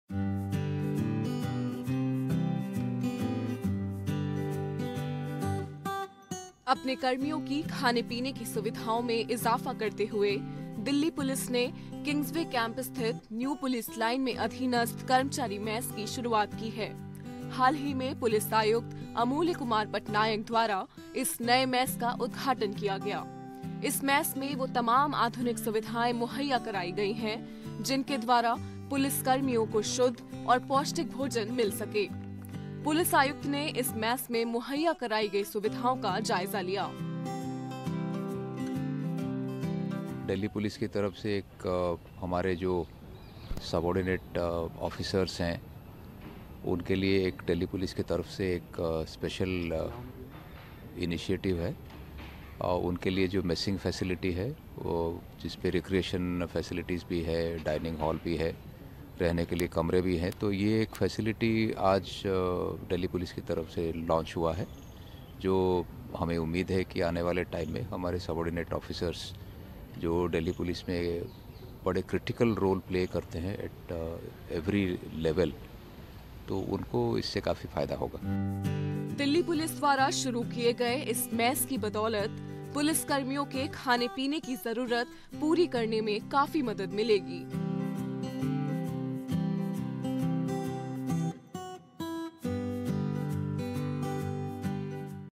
Delhi Police Commissioner inaugurates subordinate officers' transit mess, new police lines'
Delhi Police Commissioner amulya patnaik inaugurated subordinate officers' transit mess, new police lines'.